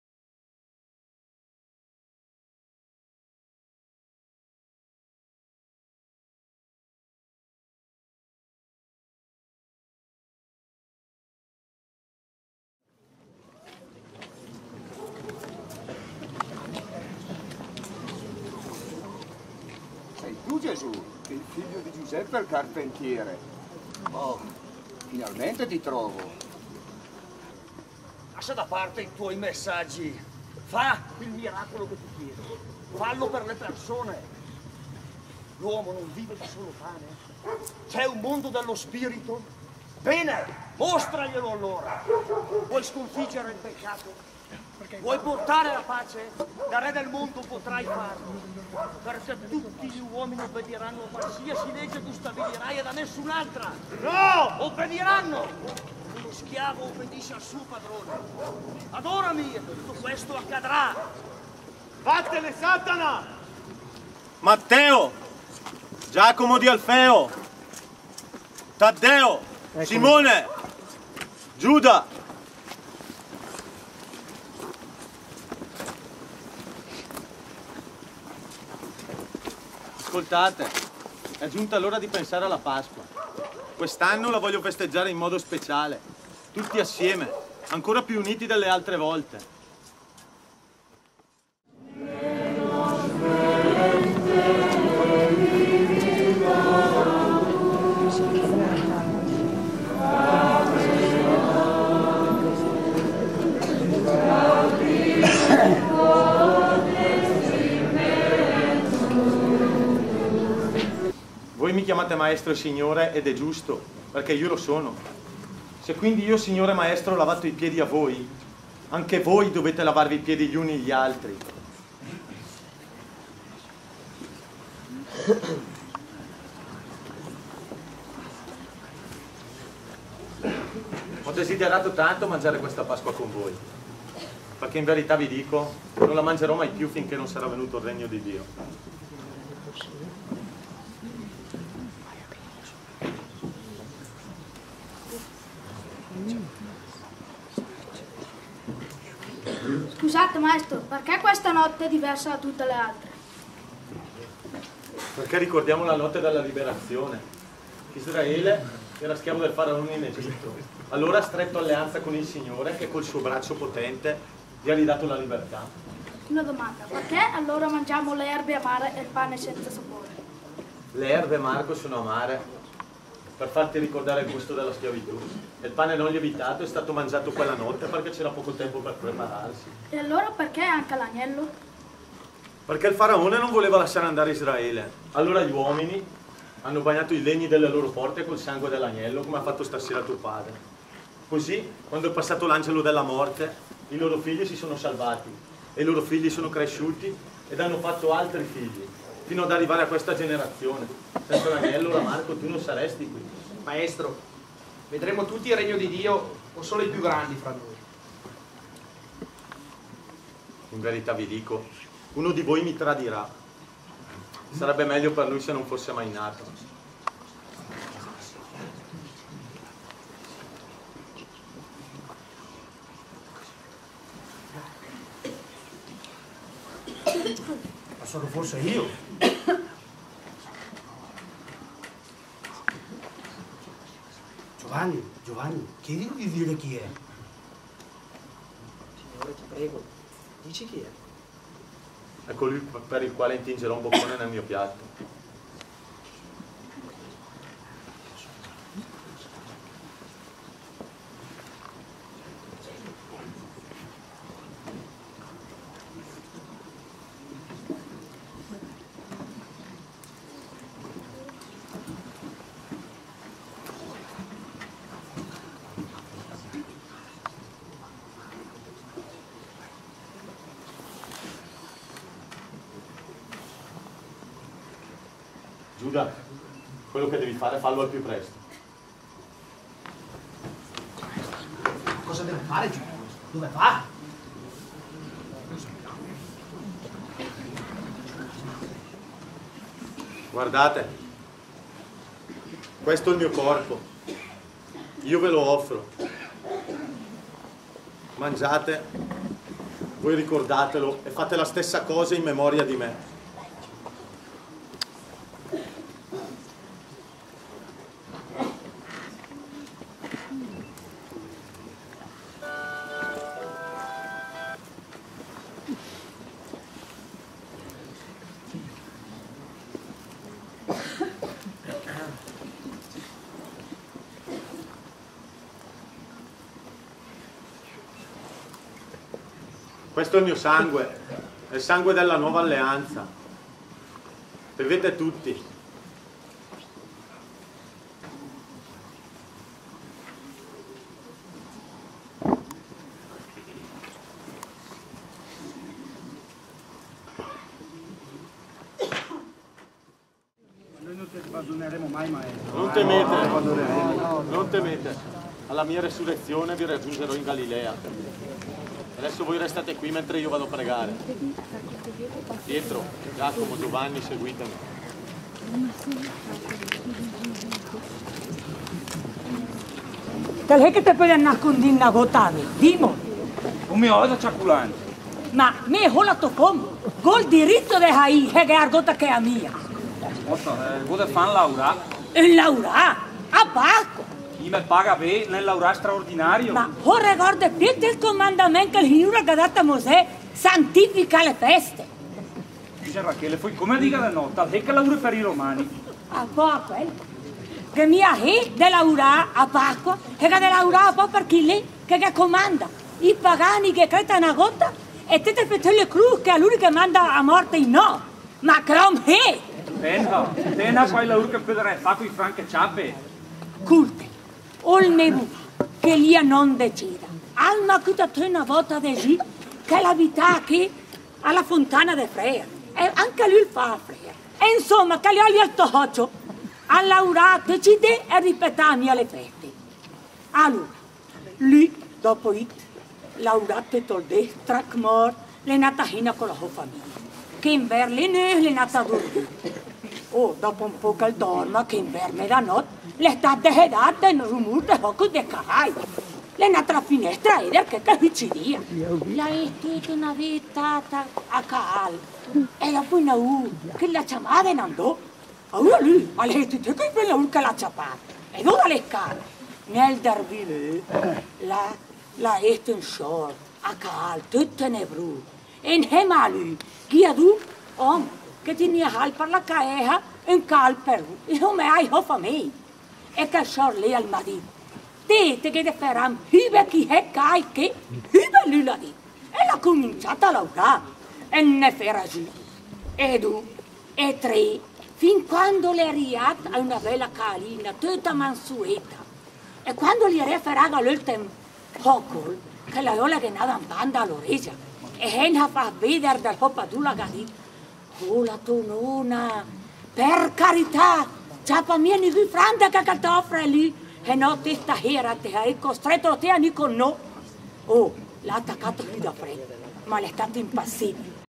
Via Sacra - Passione di Gesù a Zone (BS) parte 1/4
Via Sacra: Il legno della Santa Croce. Rappresentazione Sacra popolare della Passione di Gesù. Prima parte della Via Crucis del folkore e della cultura popolare della Comunità di Zone, in Provincia di Brescia. Antica trazione bresciana con brani di recita in dialetto bresciano.